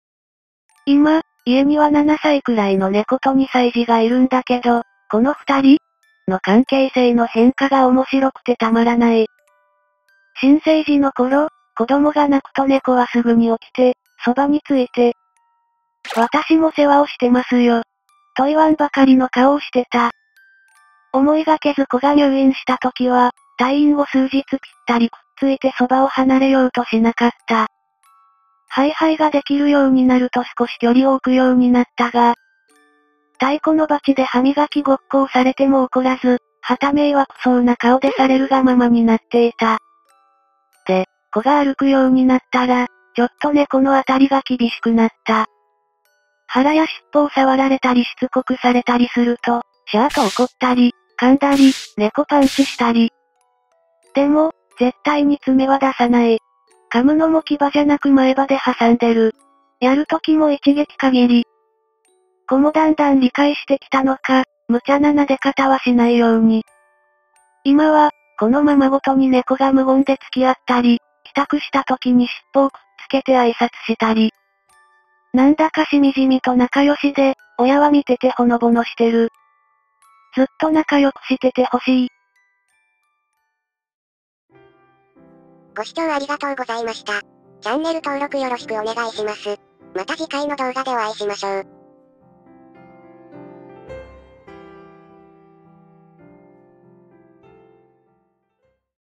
【2ch猫】我が家には人間２歳と猫７歳の２人がいる。
心温まるほのぼのエピソードを紹介します。

コメントを頂けると嬉しいです！

チャンネルで使用させていただいている素材サイト様